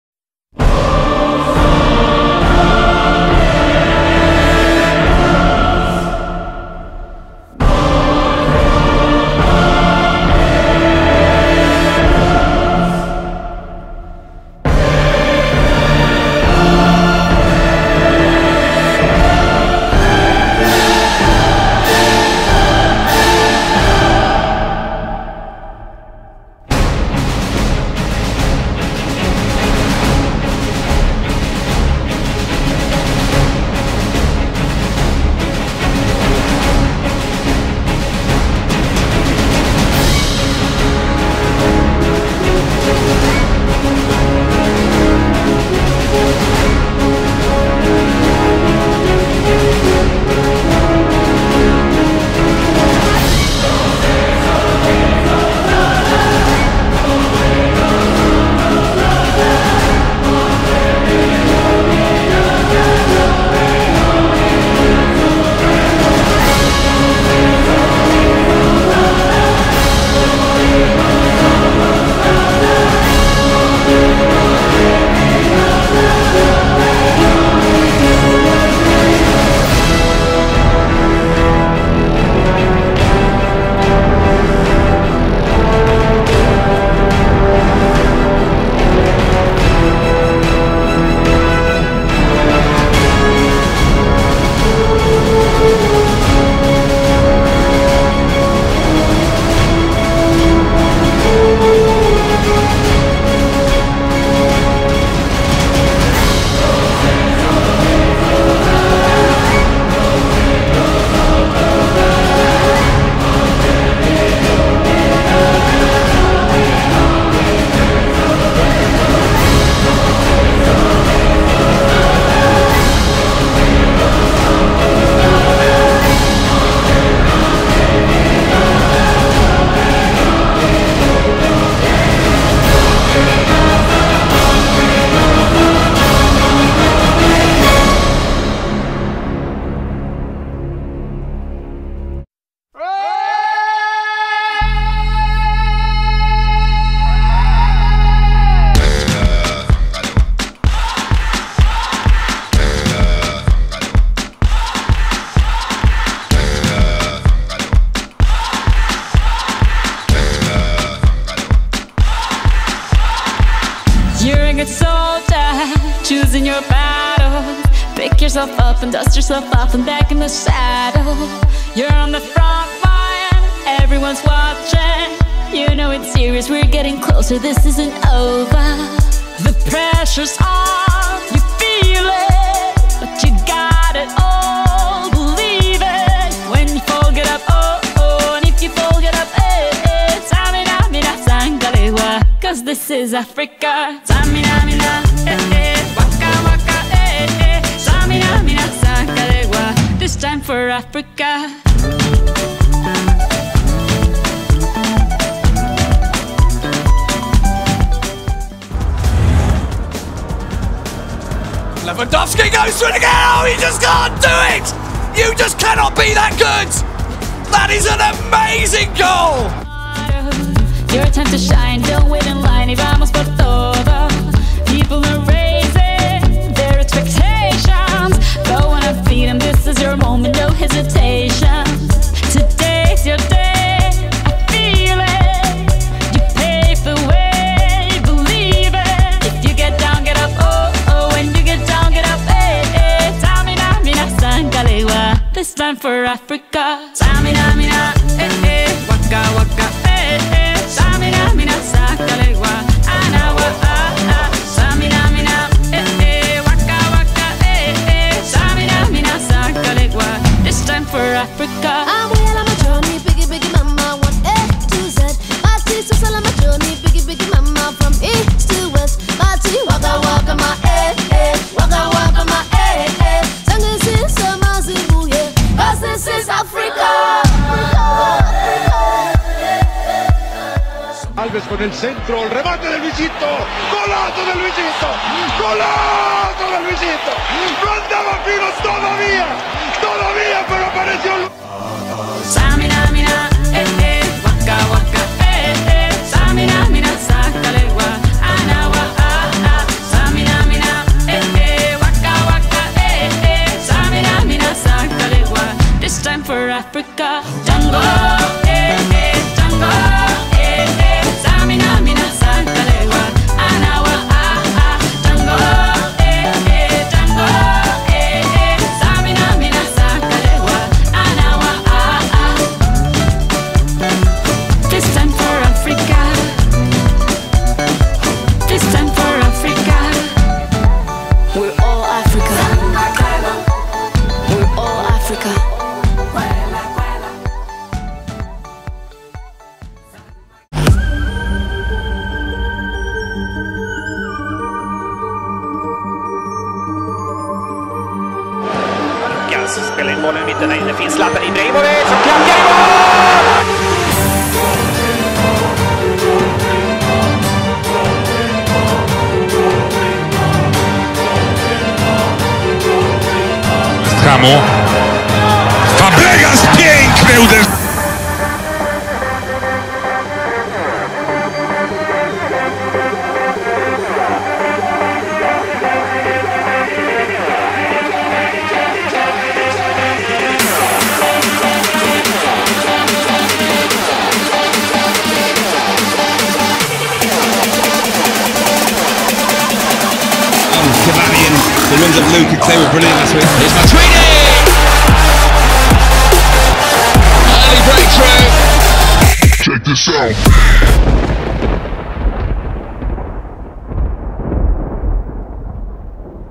Keepers Beware - Machado Edition
Compilation of skills and goals in tribute to the beautiful game!